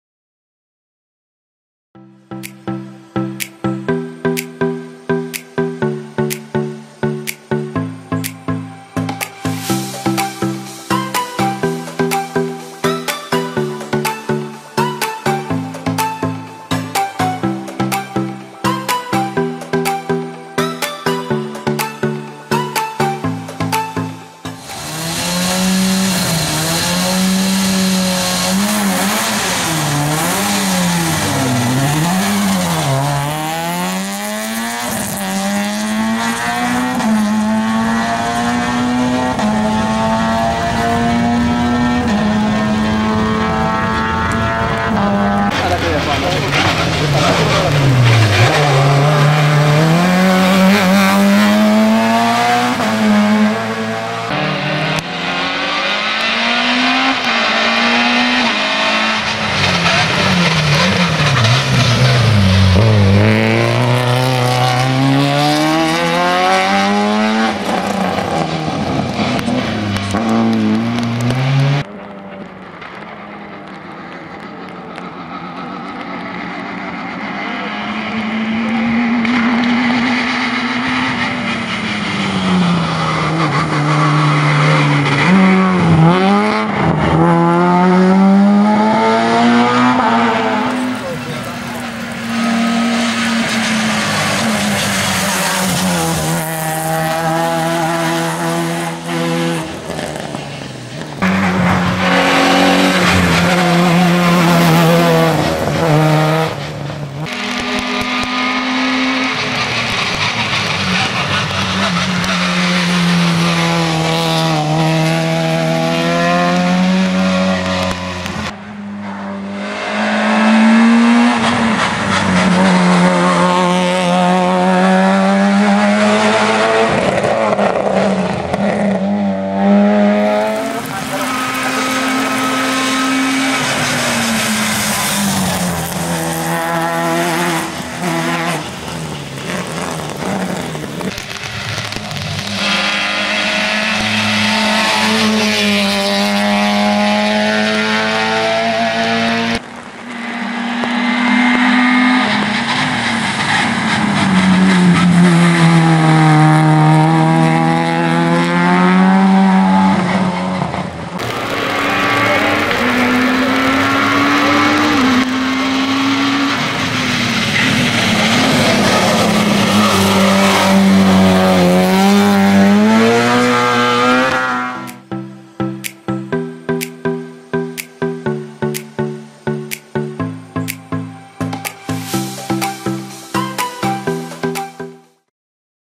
ALPE DEL NEVEGAL 2019 DAVID GRANZIERA PEUGEOT 106 S16
Una prestazione sbalorditiva è stata quella offerta da David Granziera: alla prima gara su questa vettura, l'ha guidata con grande padronanza e disinvoltura cogliendo il secondo posto di classe per appena 21/100. Da applausi!!.